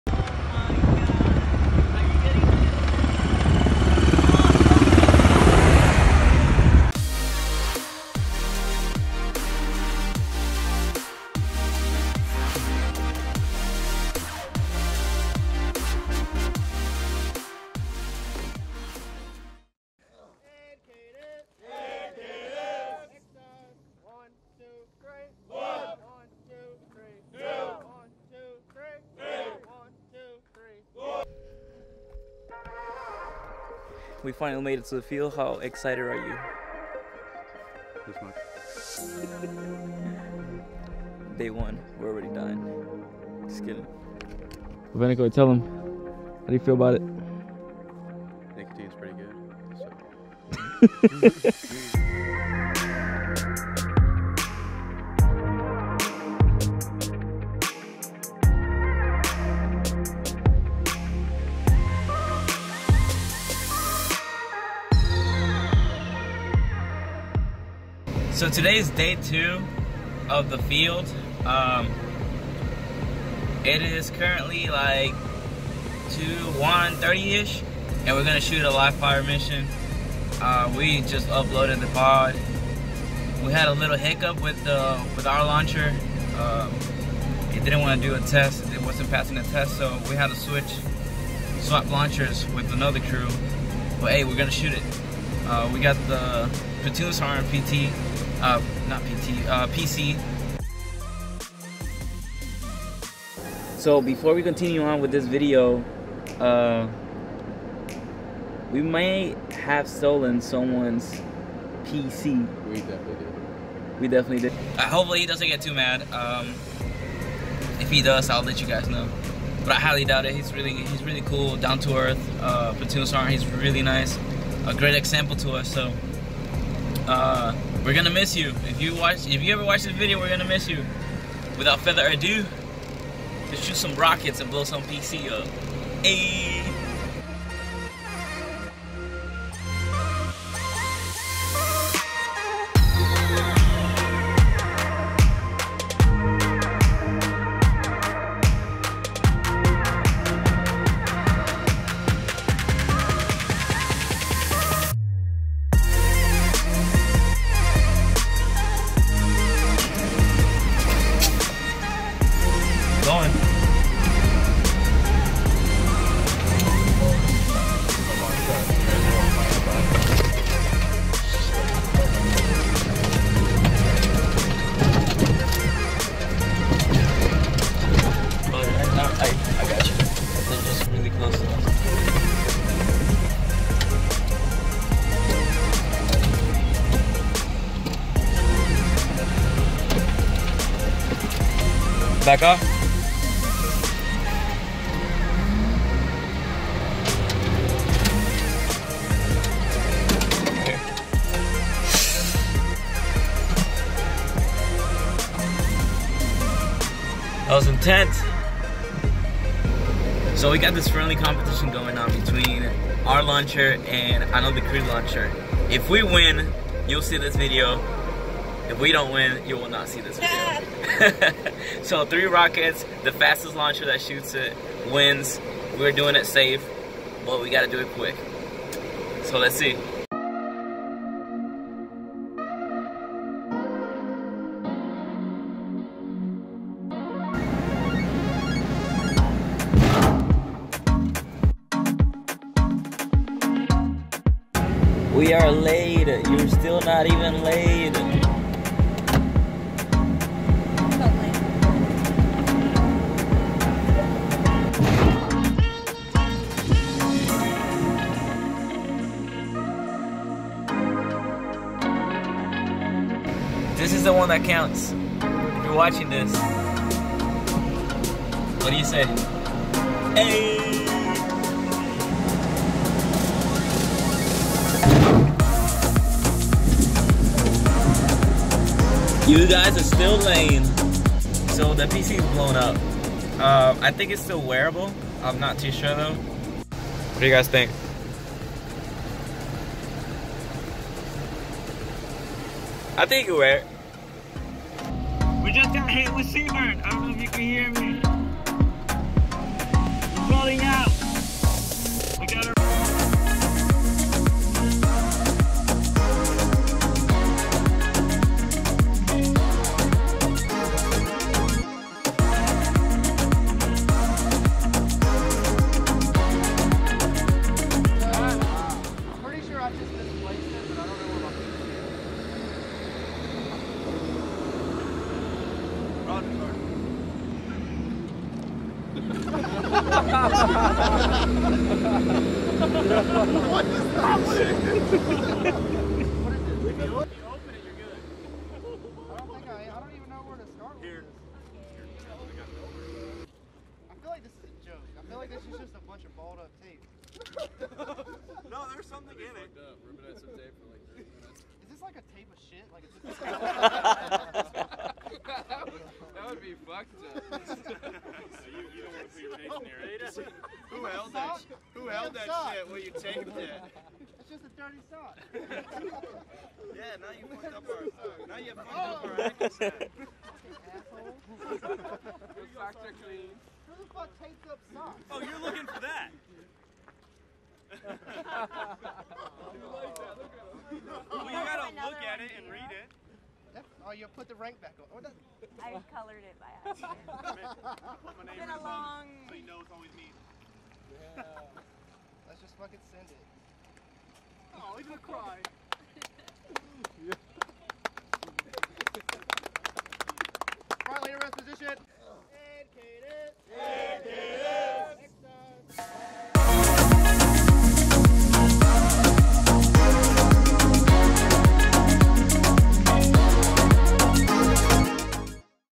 Part 2: The Vlog 13M My job
This is a Vlog of the 13M MOS in the Army. This video is for an educational purpose. This is a Vlog created to show parts of a Field Day. Disclaimer this is not how every field is, we do not record or share information that is confidential, harmful, inappropriate or illegal in any way. The purpose of this video is to inspire others and show a good time within a very serious job. Hopefully you guys enjoy, it might most likely be the only vlog I ever make of my job.